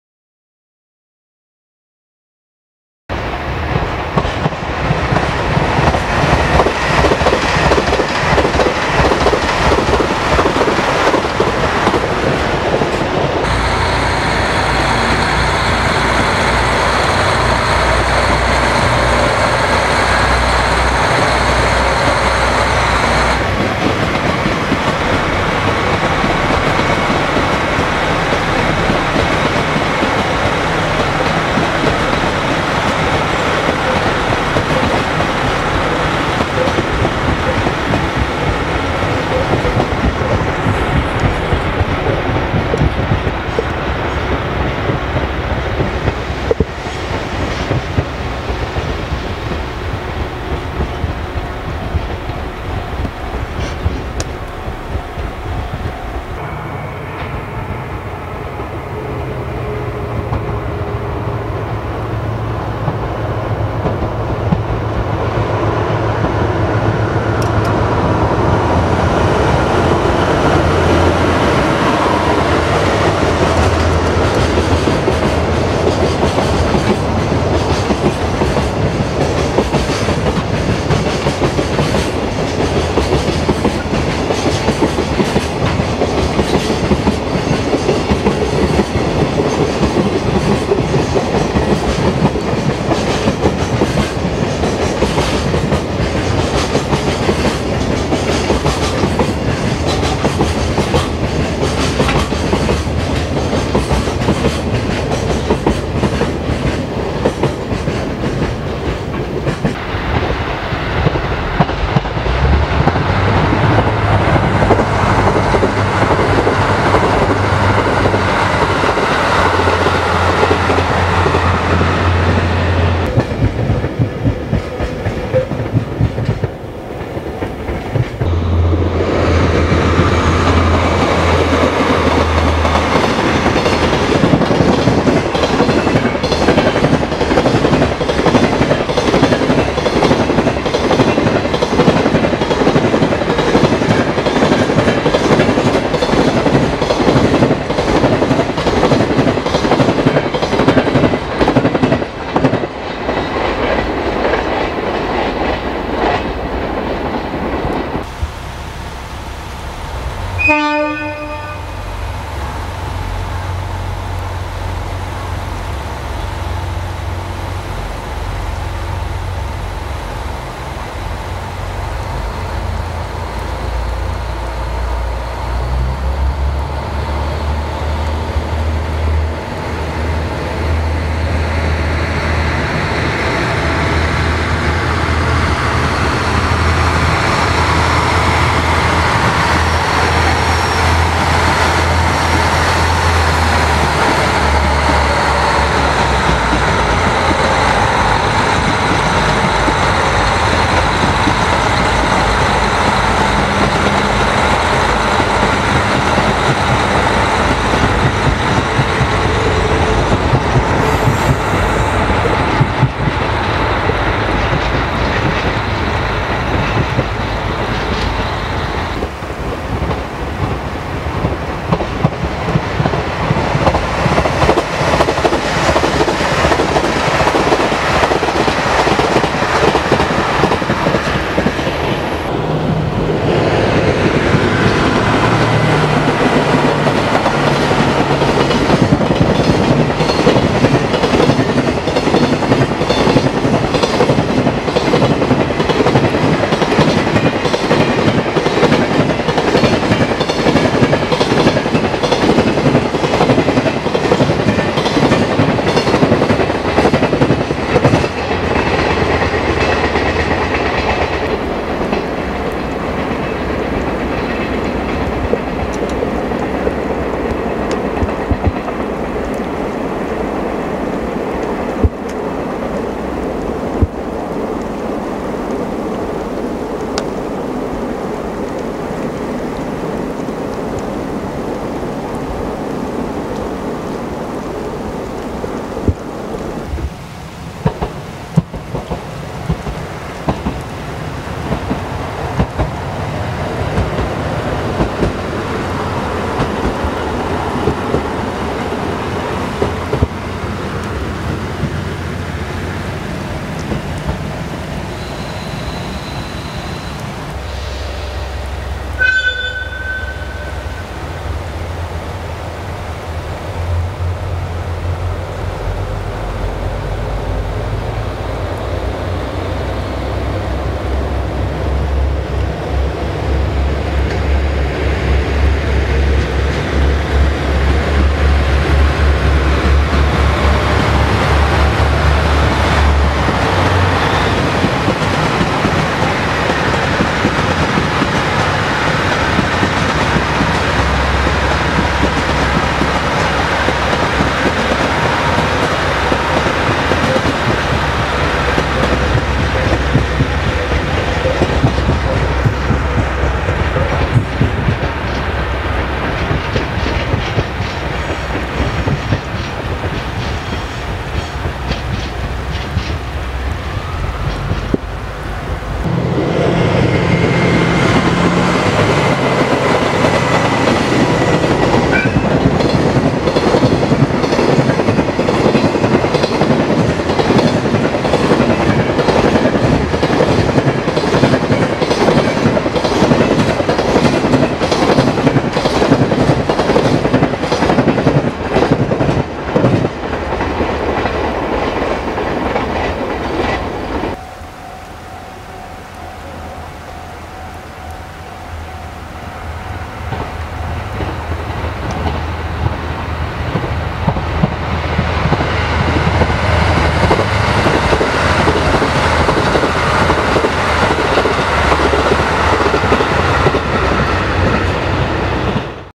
【JR北海道】2017.11.13 本日の函館本線車両色々！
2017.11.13
函館本線上り、下り色々撮影！
朝から雨模様で、頑張って働いてます。